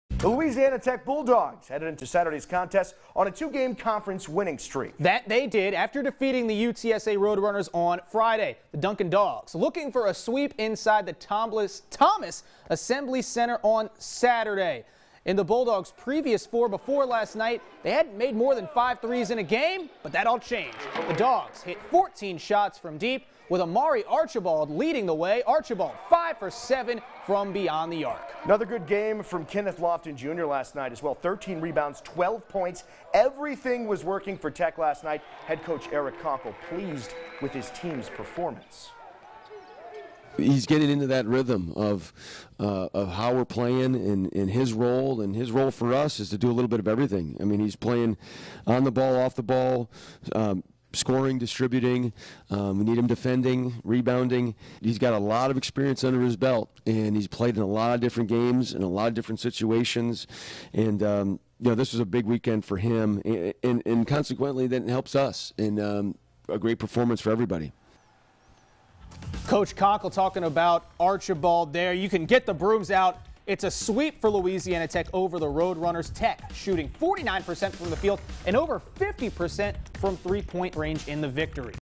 Louisiana Tech sweeps home series against UTSA
Bulldogs drain 14 three-pointers in victory over Roadrunners